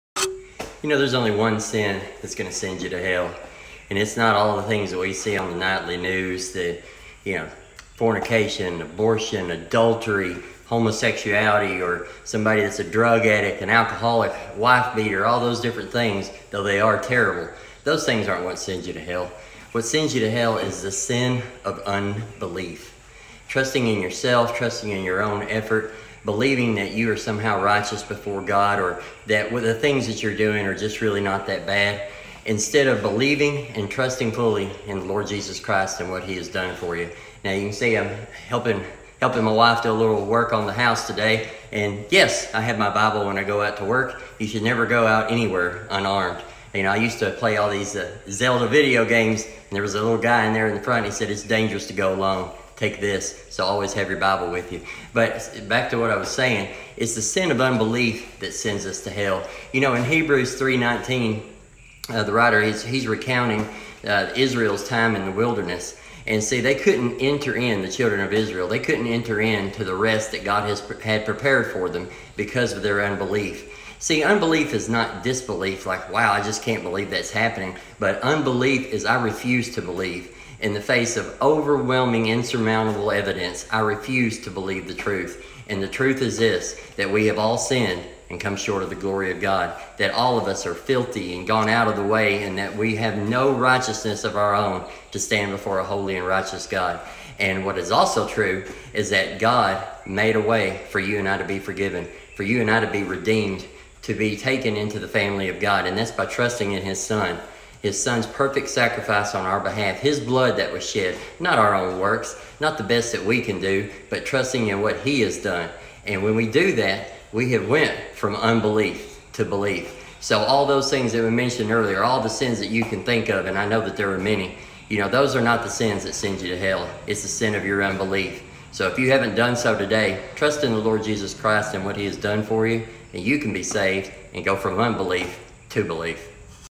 𝐓𝐡𝐞 𝐬𝐢𝐧 𝐭𝐡𝐚𝐭 𝐬𝐞𝐧𝐝𝐬 𝐲𝐨𝐮 𝐭𝐨 𝐡𝐞𝐥𝐥
It's not pornography, fornication, or even murder; it's the sin of unbelief.  What is unbelief?  Refusing to believe in the face of evidence to the contrary, or trusting in ourselves rather than God.